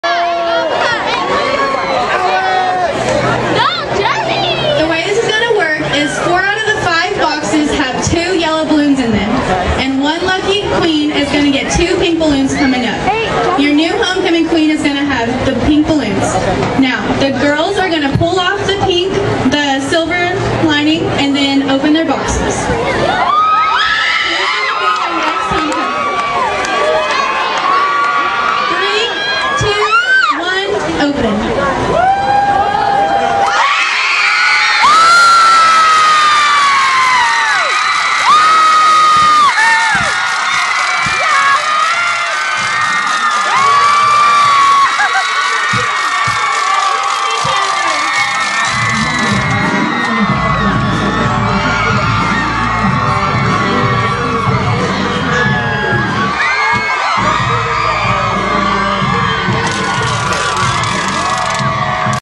Golden West Homecoming Queen 2012
Kelsie Hendrix with her fellow nominee Alexandra Keyes and their reactions when Alex won. So awesome!! Kelsie was so excited for Alex!